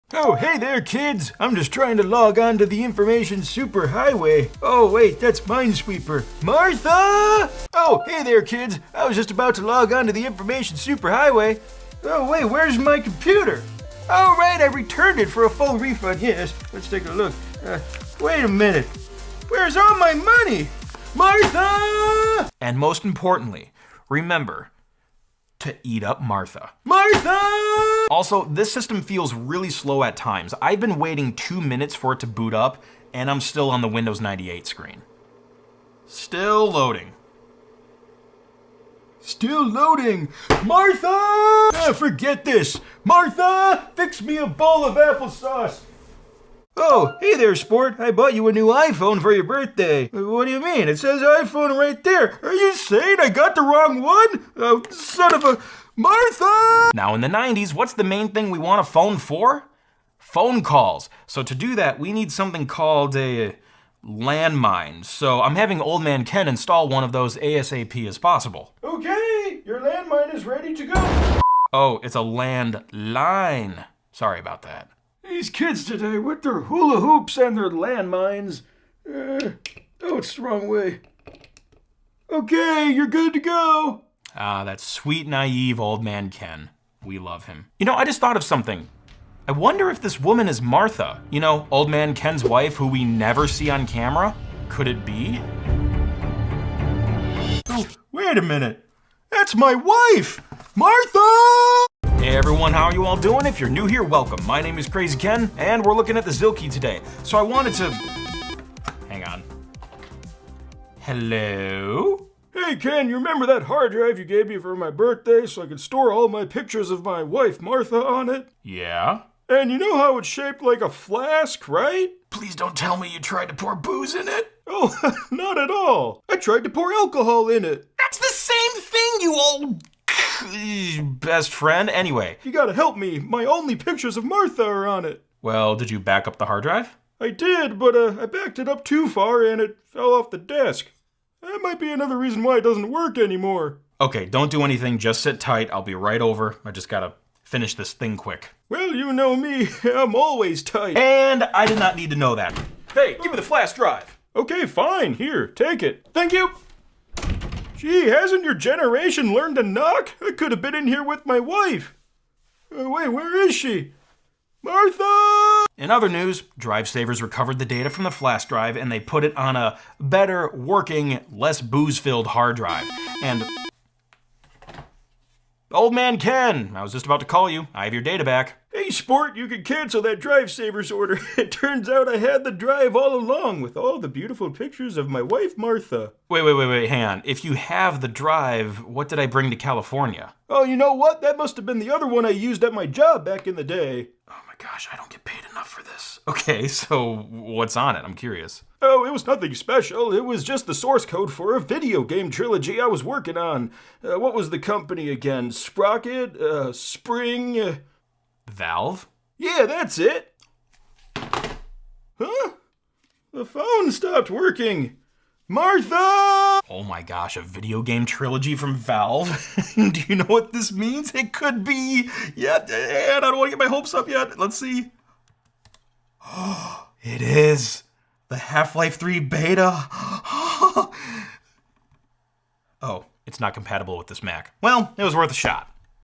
All Old Man Ken Moments (Computer Clan)
Ever wanted to hear all of Old Man Ken's ramblings, all in one place? You're in luck!

0:00 I Bought a $1,300 Computer Made for Seniors
0:07 My $1,300 "Senior Computer" (I Want My Money Back!)
0:22 Unboxing a SEALED Newton MessagePad 100 (After 28 Years)
0:28 Illegal iMac Knockoff: The U.S. Banned This PC [Bootup]
0:41 Illegal iMac Knockoff: The U.S. Banned This PC [Outro]
0:46 Apple Wanted This Product Name BADLY [Intro]
0:56 Apple Wanted This Product Name BADLY [Setup]
1:27 Is ThePhotoStick a Scam? (Nope! But…) [Wife]
1:37 Is ThePhotoStick a Scam? (Nope! But…) [Outro]
1:42 Zilkee Recovery Converter is a SCAM! [Intro]
2:32 Zilkee Recovery Converter is a SCAM! [Give Me The Flask Drive]
2:45 Zilkee Recovery Converter is a SCAM! [Conclusion]